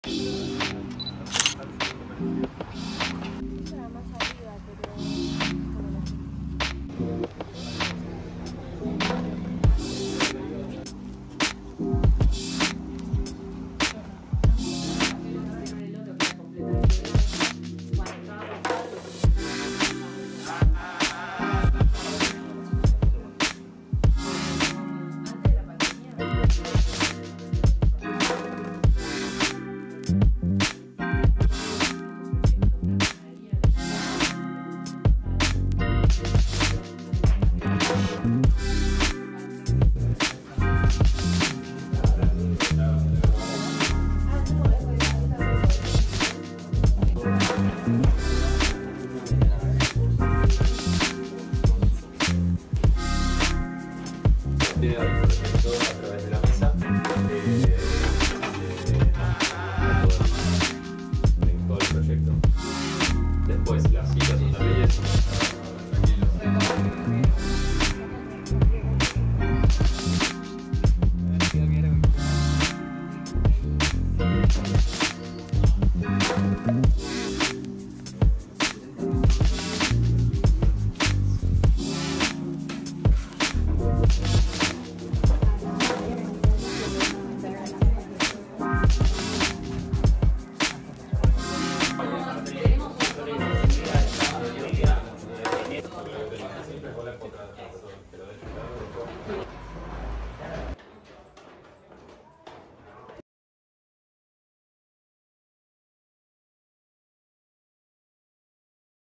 Open House Bs.As. - Visitamos el Edificio Cervantes (Grizzo Estudio)
Hola!, como estan todos, soy alcon.ph y en este video visitamos el Edificio Cervantes (Grizzo Estudio), ubicado en el barrio de Villa Devoto